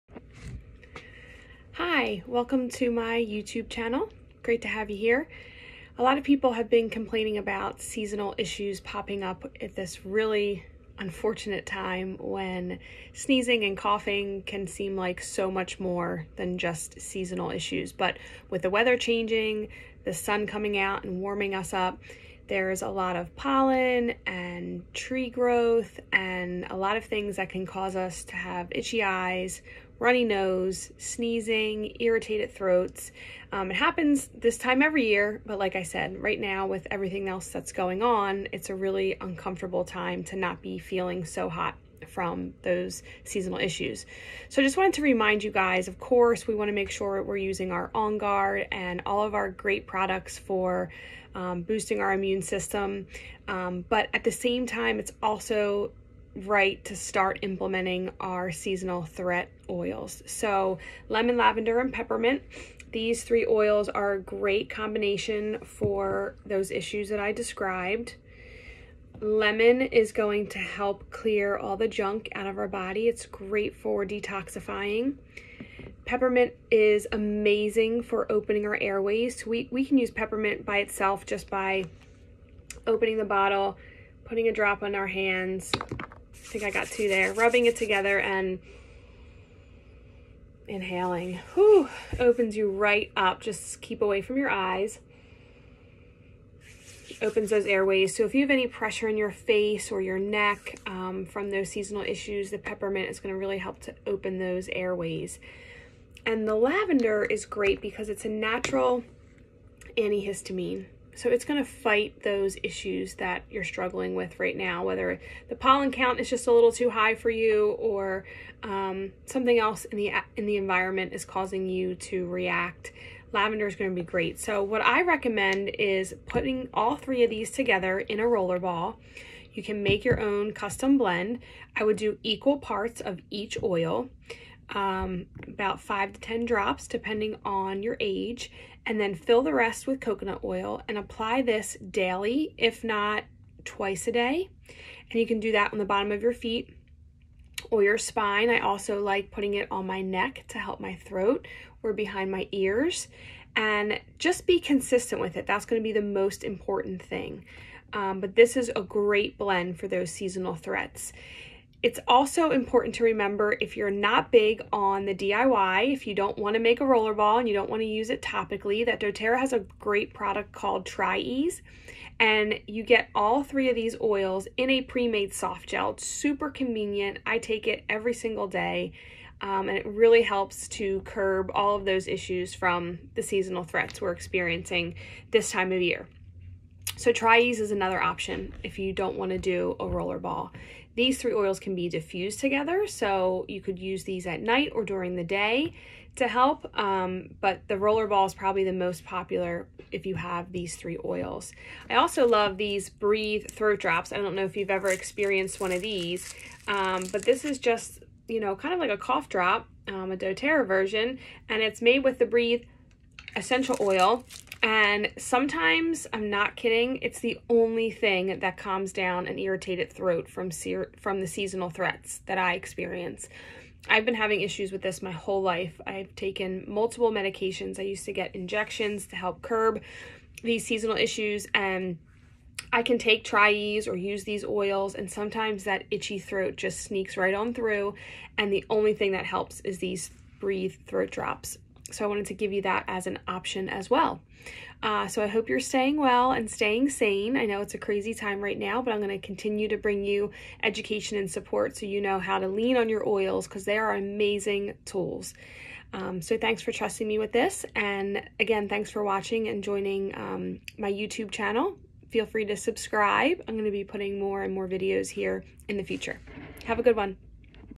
It's Spring and I'm sneezing!
Learn which essential oils can support your seasonal issues as Spring is upon us!